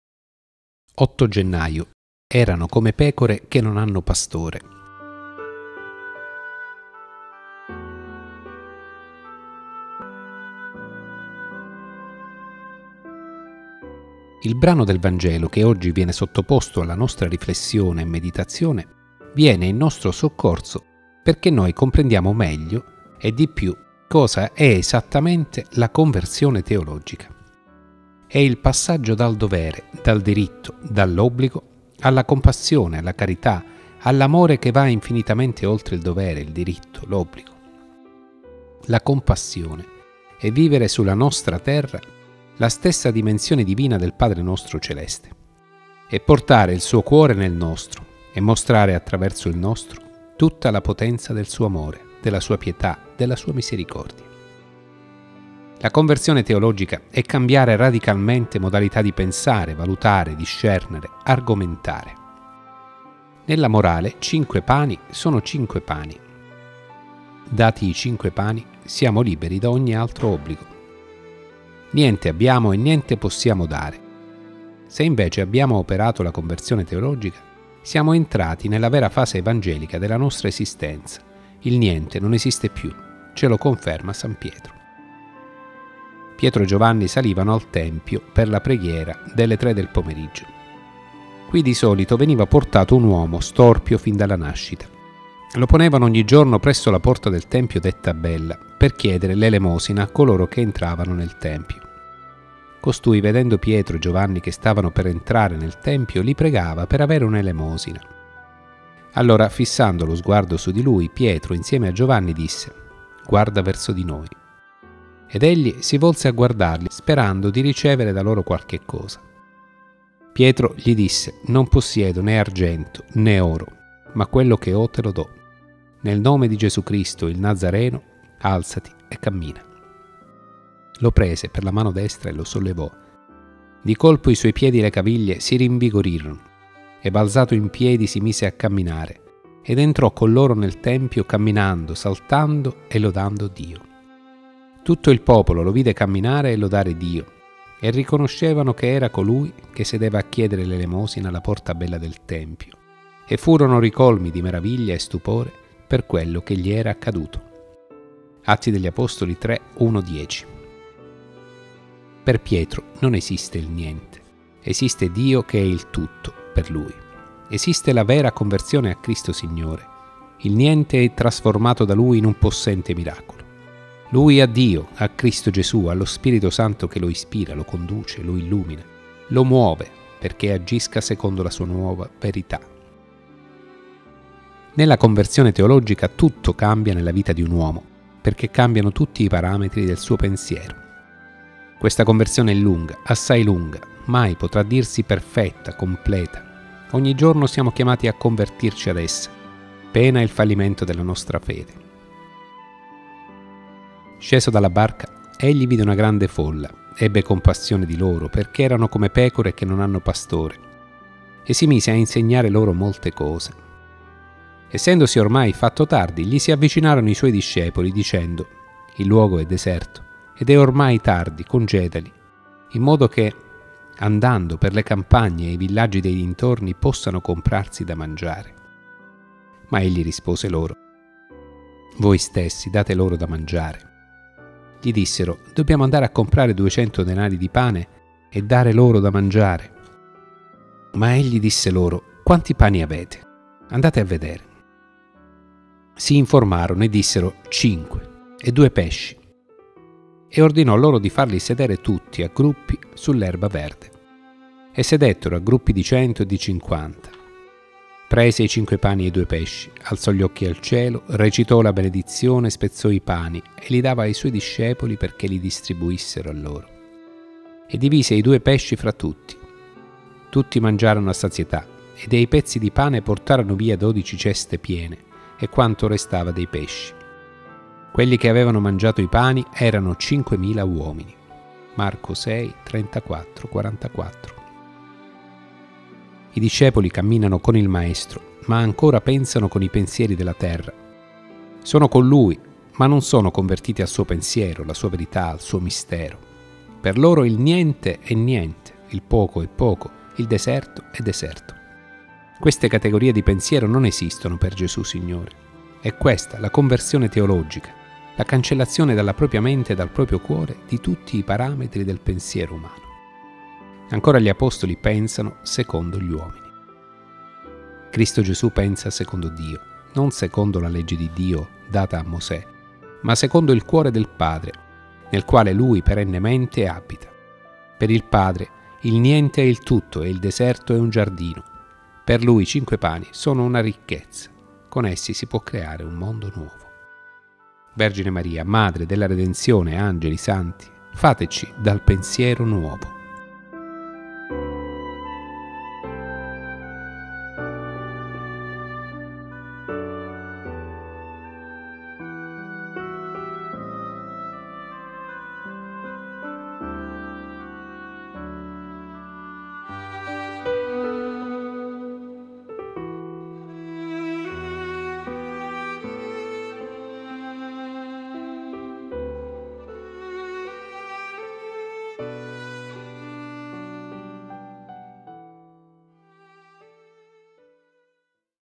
Il vangelo del giorno 8 Gennaio 2015
A cura del teologo Mons. Costantino di Bruno nostro Assistente Ecclesiastico